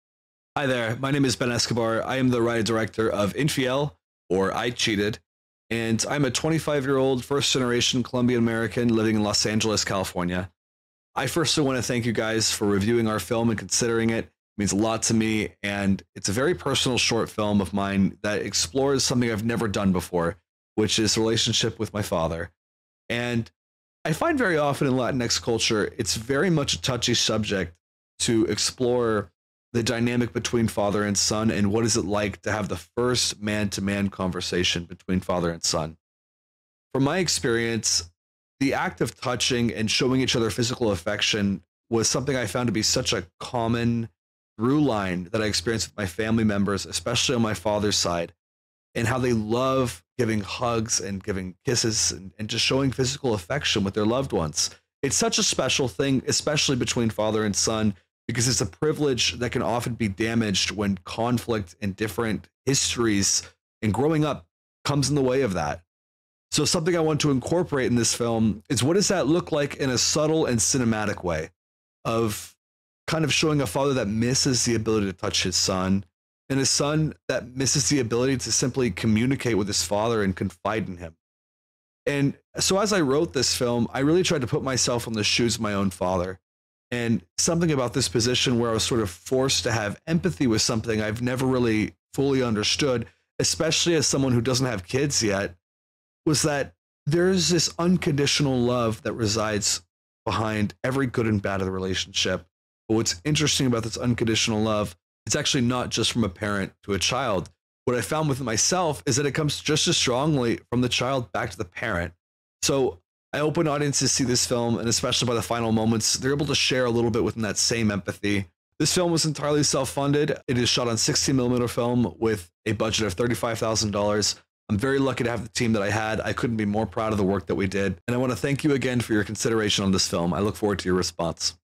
Cine Pobre Film Festival contributors
meet 2023 cinepobre contributor and director of INFIEL Ben Escobar